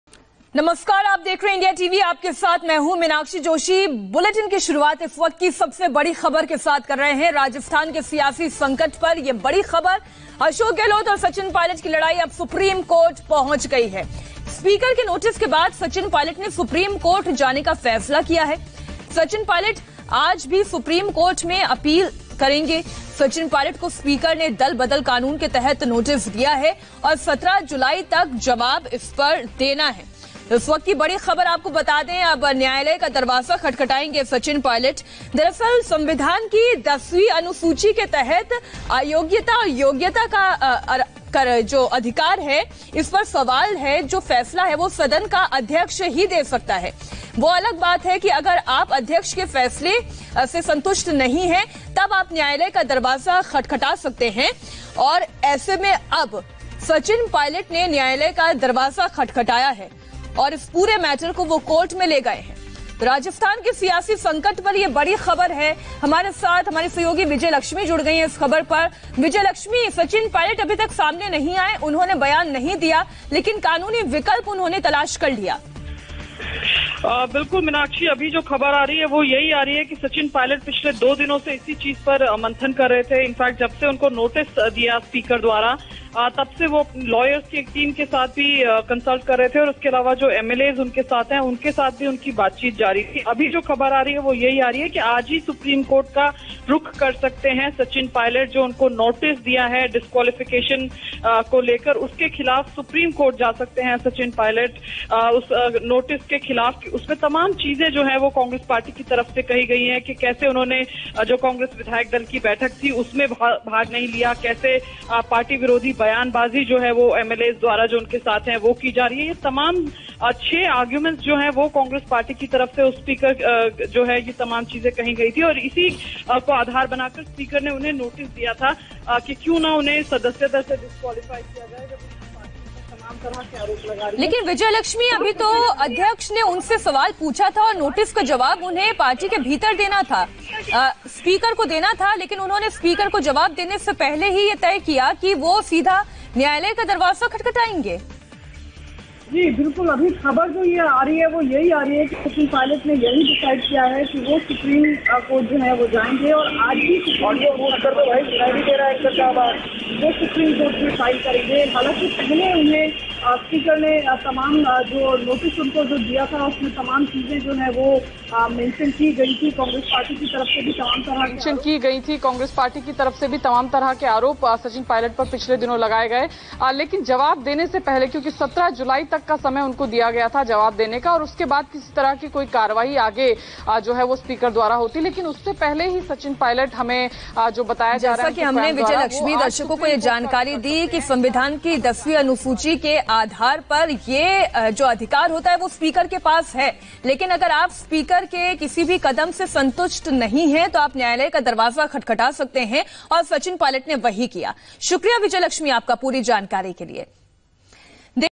स्पीेकर के नोटिस के खिलाफ SC जाने की तैयारी में सचिन पायलट, वकीलों से कर रहे हैं चर्चा
राजस्थान कांग्रेस में जारी सियासी बवाल फिलहाल थमता नजर नहीं आ रहा है। बुधवार को राजस्थान विधानसभा के स्पीकर ने सचिन पायलट सहित 19 विधायकों को दल बदल कानून के तहत नोटिस भेजा है। स्पीकर के इस फैसले के खिलाफ अब पायलट सुप्रीम कोर्ट जाने की तैयारी कर रहे हैं। पिछले दो दिनों से पायलट अपने वकीलों से इस संबंध में राय ले रहे हैं। सचिन पायलट को 17 जुलाई तक विधानसभा अध्यक्ष के नोटिस का जवाब देना है